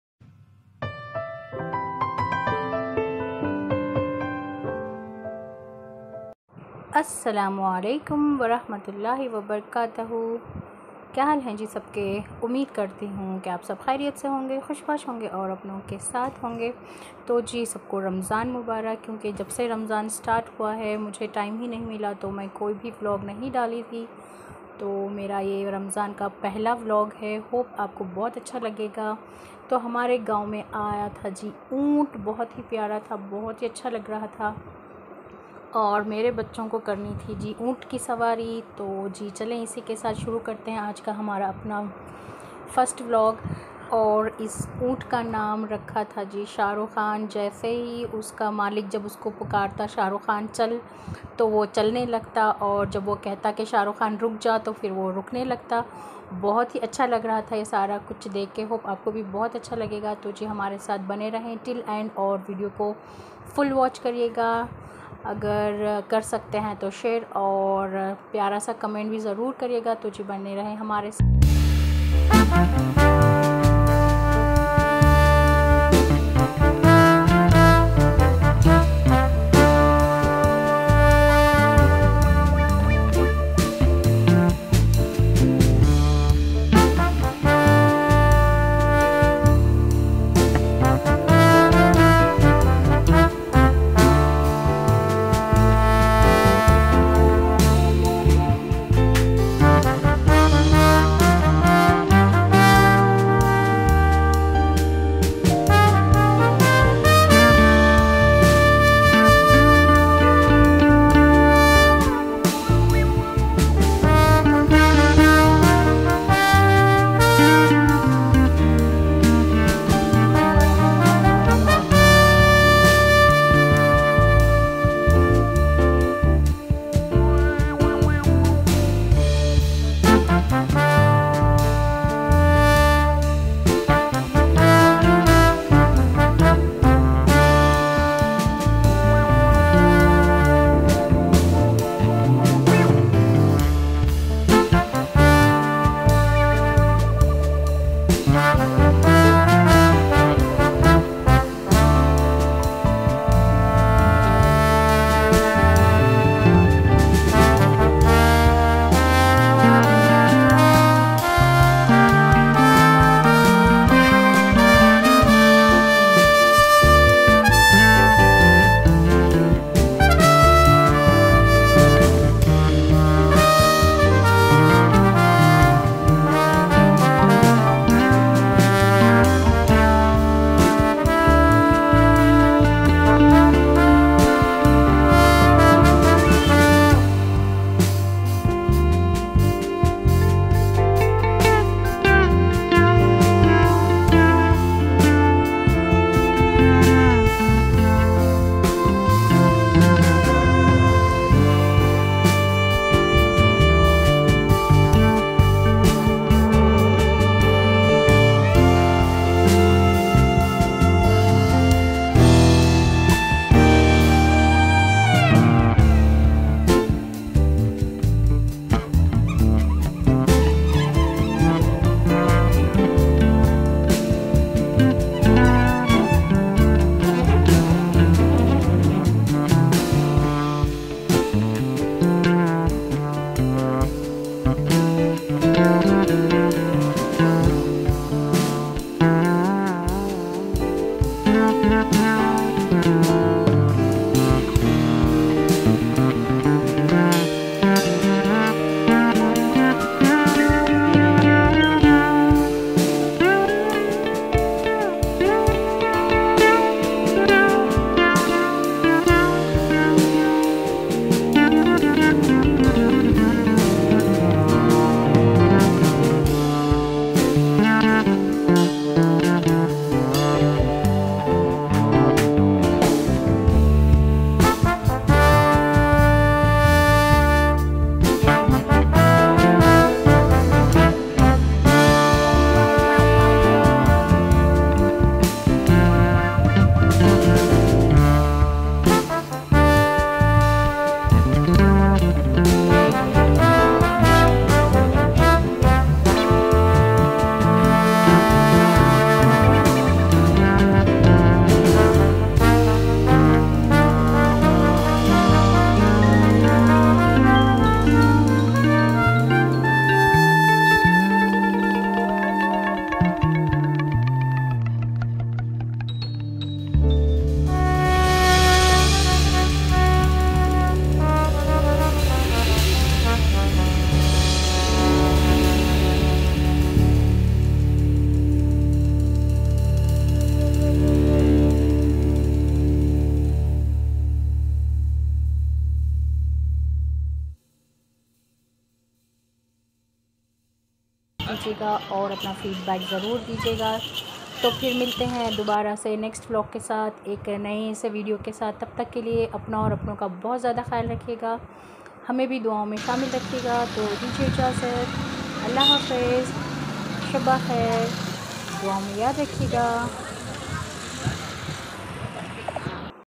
My First Vlog In Ramazan 🐪 Camel Ride 🐪 Kids Fun And Masti
assalamualaikum kya haal hai sab ke sab ko Ramazan Mubarak ho allah pak hum sab ke jayz duao ko khubool farmaye ameen sumameen ❤